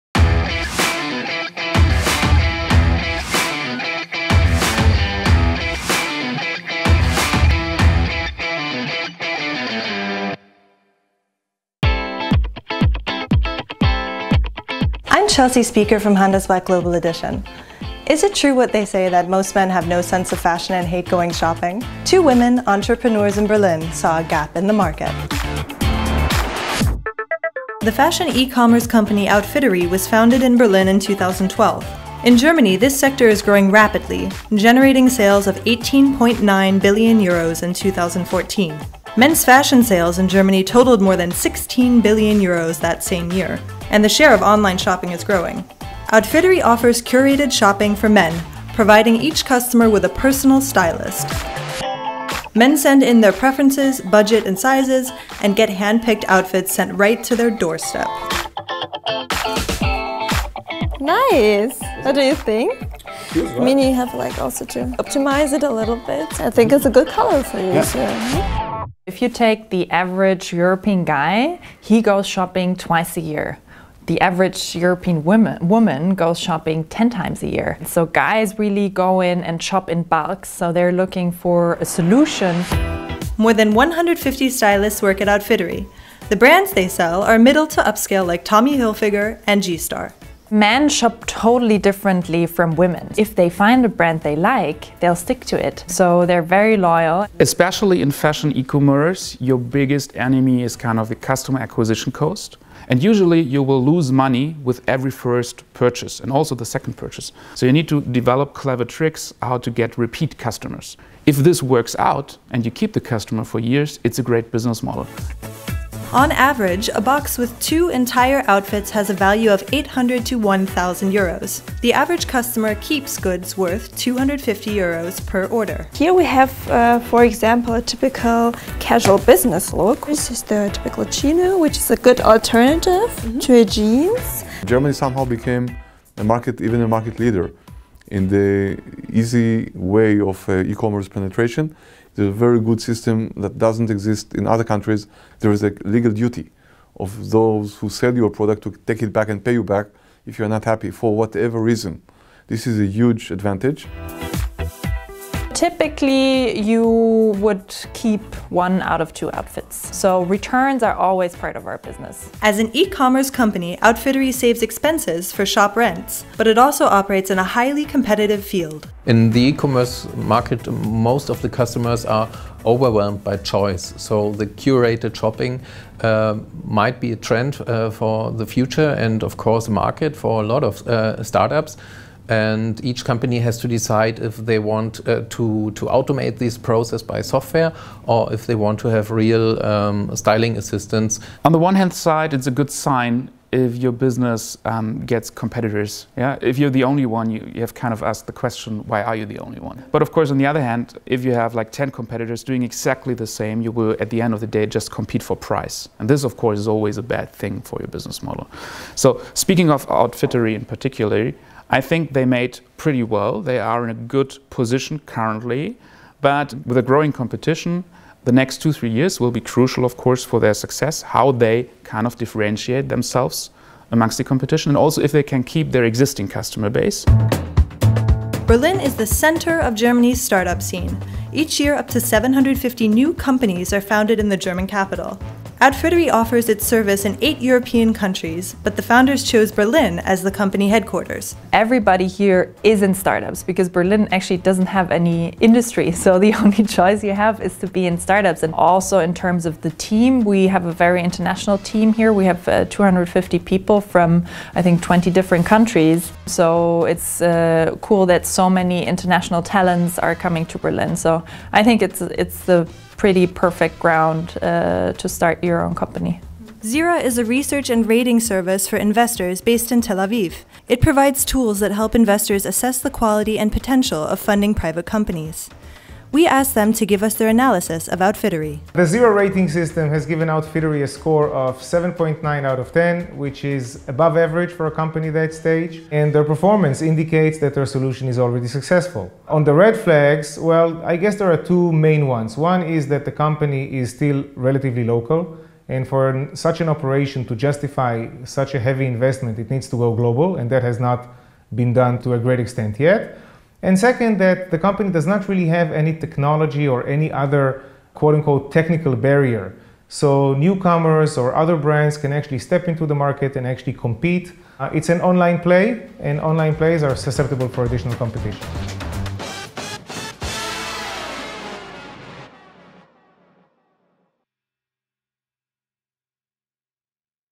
Berlin’s Startup Scene #1 Outfittery
As the capital of a country known for world-class industrial strength, Berlin is Germany’s famous laggard in terms of capital and labor. But what it lacks in traditional industry, it makes up for with innovative startups. Our special video series takes a look at eight companies helping to transform the “poor but sexy” city into a darling of Germany Inc. Part One takes a look at Outfittery, a startup focused on changing the way men shop for their threads.

participants:
Julia Bösch, co-founder, Outfittery
Nikolas Samios, COO, German Startup Group
Thomas Bendig, Research coordinator, Fraunhofer Information and Communication Technology Group
Eran Davidzon, Founder, Davidzon Technology Growth Debt Fund

Credits: Series Editor: Tal Rimon; Presenter, Narrator, Researcher: Chelsea Spieker; Video Editor: Guy Raphael; Film Director: Steffi Cassel; Camera: Jens Grumpelt, Justin Morris (Outfittery and Ascribe), Graphics: Christoph Schmid